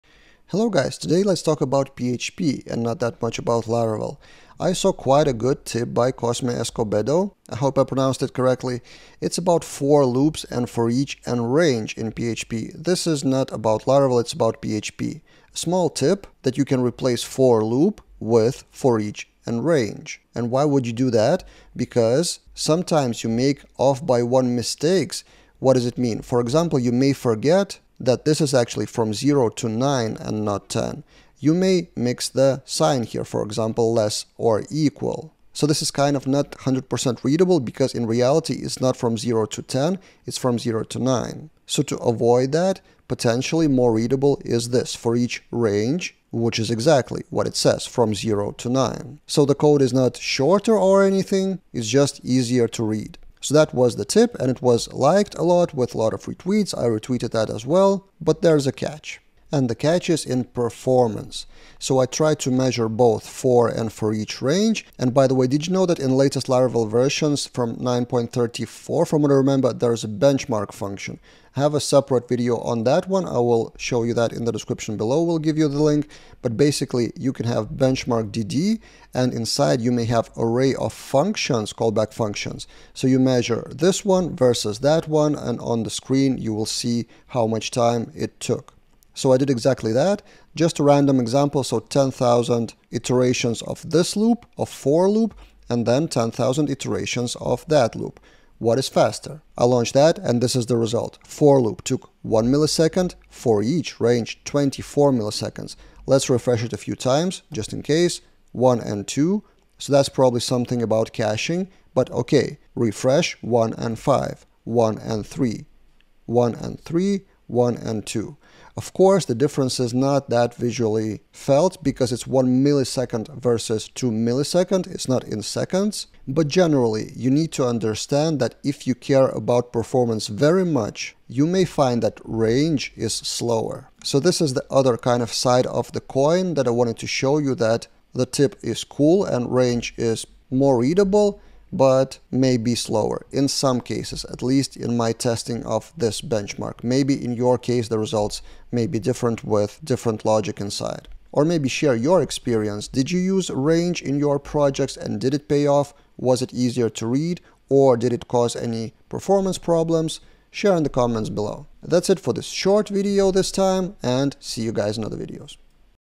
PHP Tip: Replace For Loop with Foreach & Range
Quoting a tip I've seen on Twitter, sometimes the range() method may be more readable than for() loop. But is it faster?